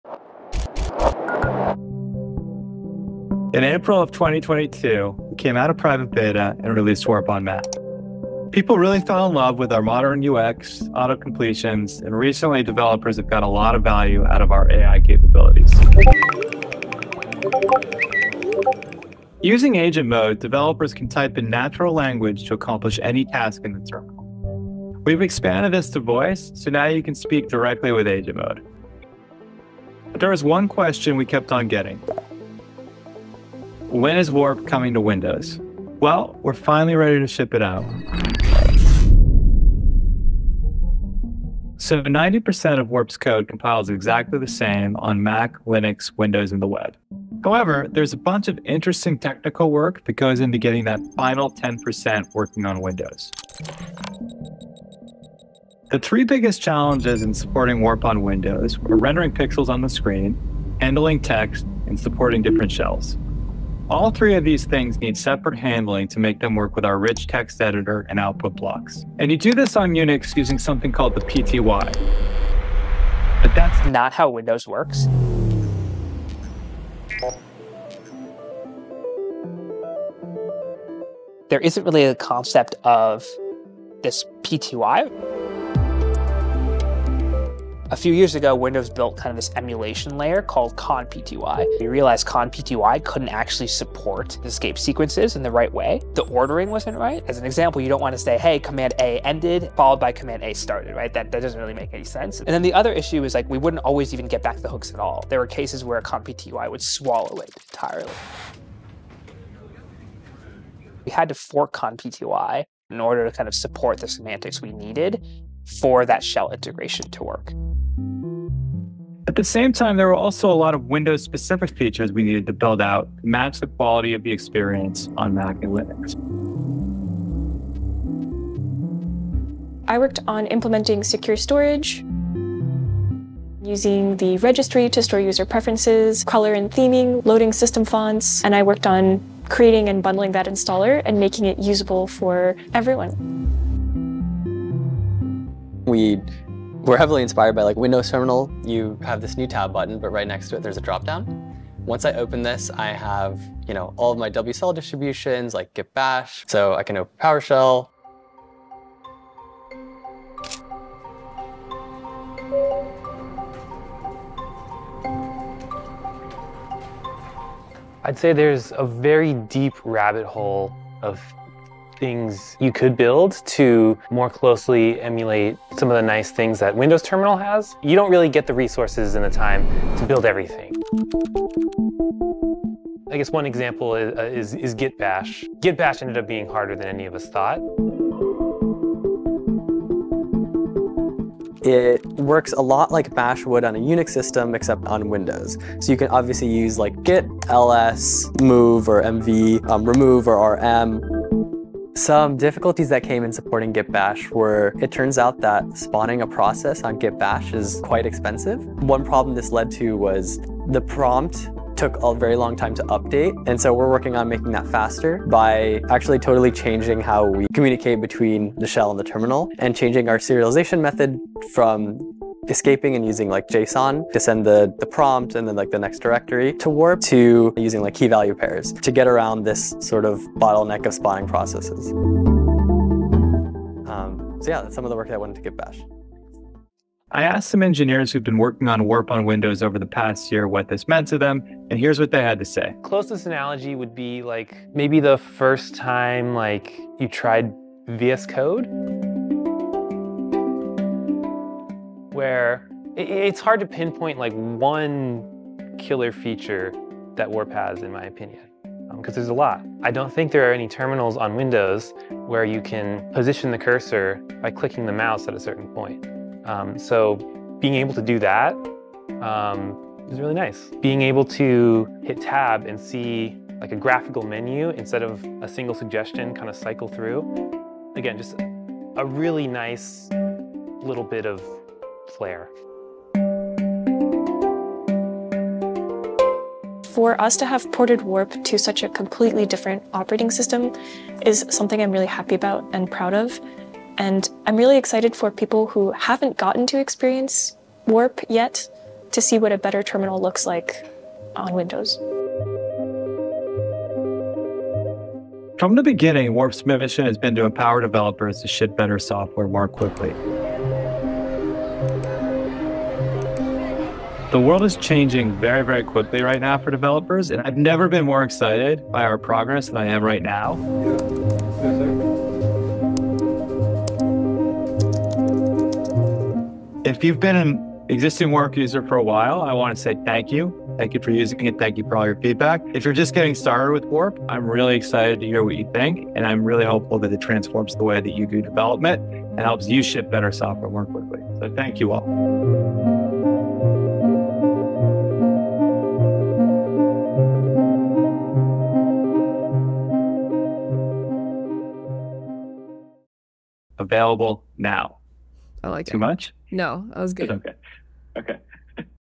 Warp on Windows...has arrived
TODAY IS THE DAY. WE ARE FINALLY LAUNCHING WARP ON WINDOWS!!!

It's been a huge effort over the past year from the engineering team to get this over the line, and we're so excited to share our story with you :-)

TIMESTAMPS
0:00 The Beginning of Warp
0:31 Wen Warp on Windows?
1:12 Problems with the PTY (ConPTY)
1:58 Building Core Windows Features
2:48 Trusted Tester Program and Multi-Shell Support (Git Bash)
3:56 Messages from the Engineers
5:04 A Final Message From Our CEO